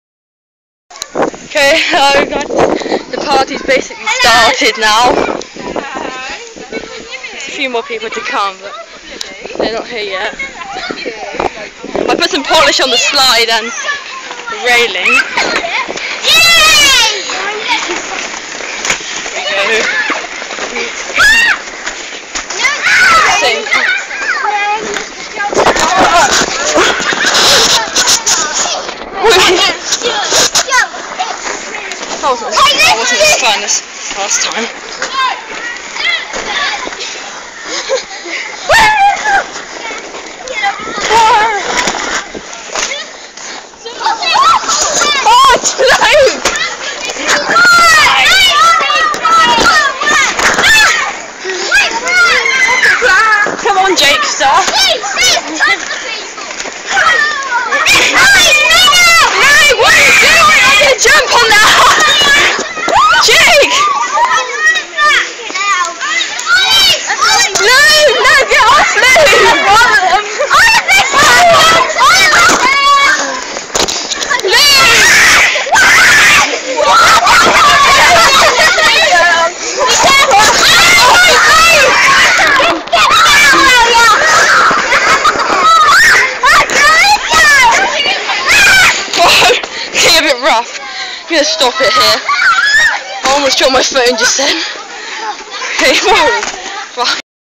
My bros party
Vidio of everyone at a party on a bouncy slide